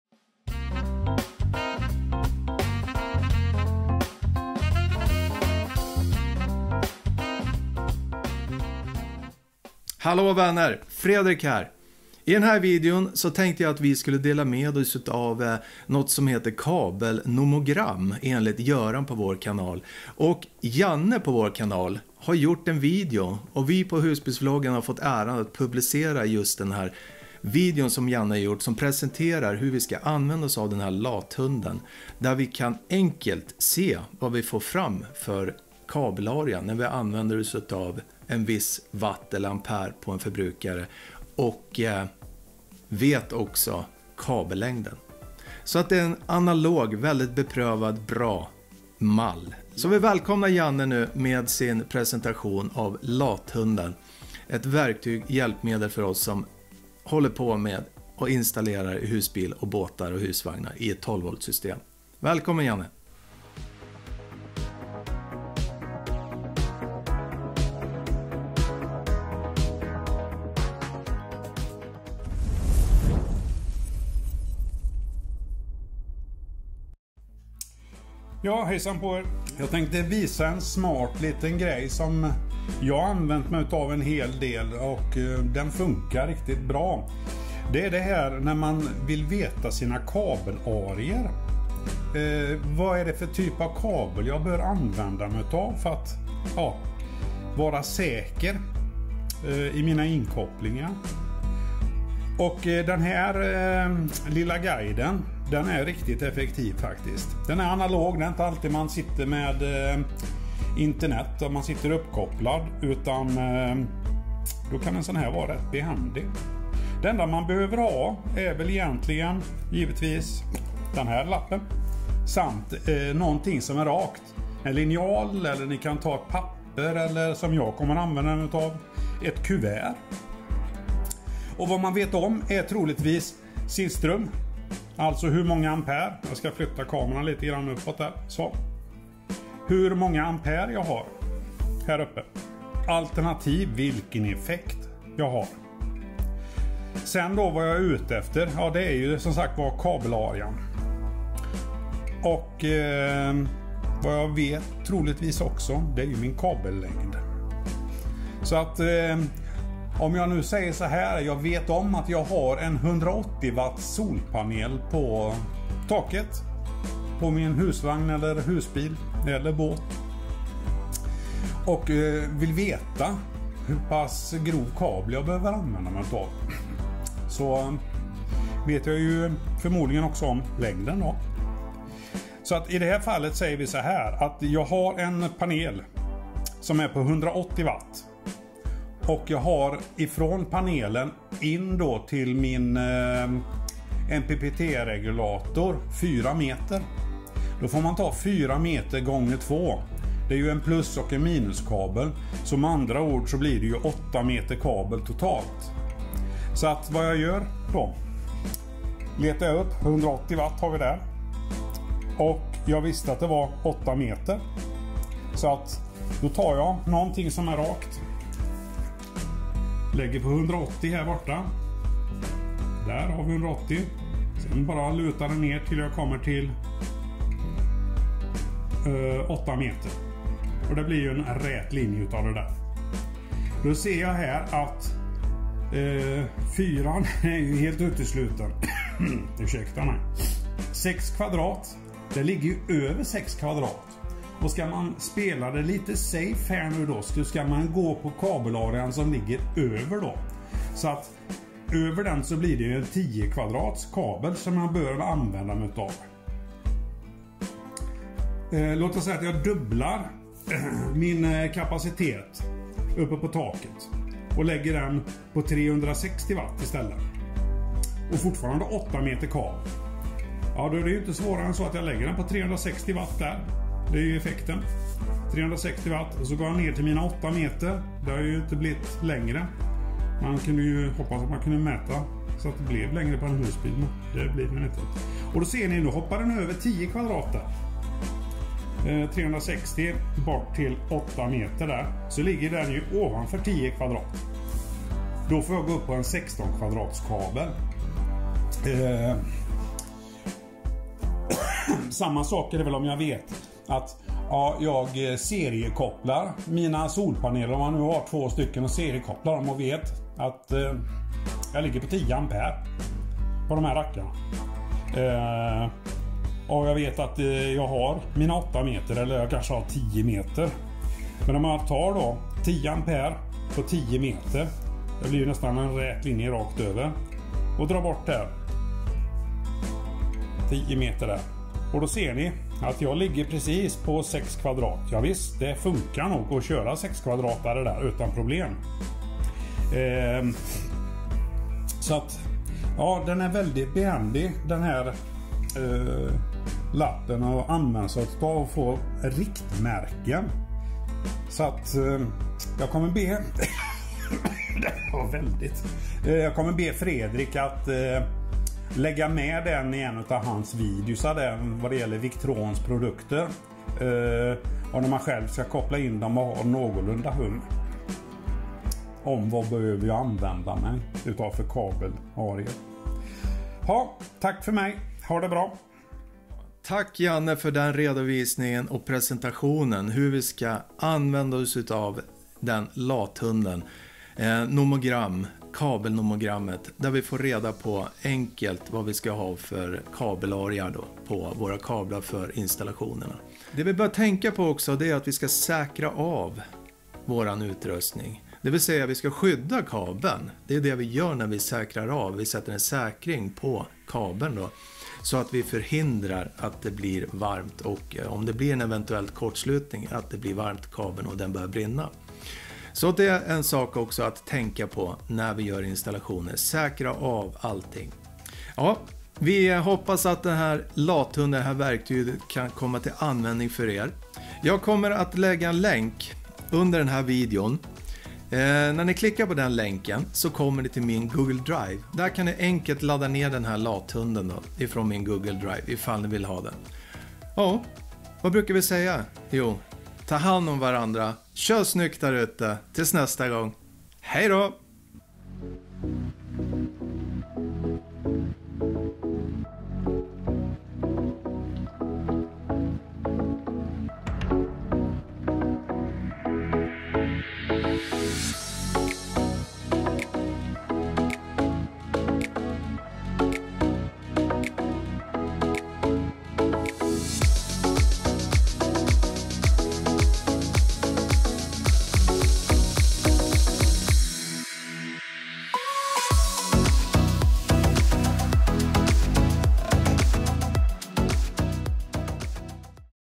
Hur Du Enkelt Väljer Rätt Kabelarea För 12 Volt Installationer
Det här verktyget och hjälpmedel "lathund" är väldig behändig när vi ska ta fram rätt kabelarea för 12 Volt installationer i vår Bil, Husbil, Husvagn eller Båt
Klicka här för att ladda ner denna Kabel Nomogram "Lathunden":

Upptäck Varandra Med Tryck Från Husbilsvloggen, Känn Att Ni Är Med I Gänget 😀

Ha en fin dag och kör snyggt 🤗
Med Vänliga Hälsningar,
Fredrik & Eleonor och Gabriel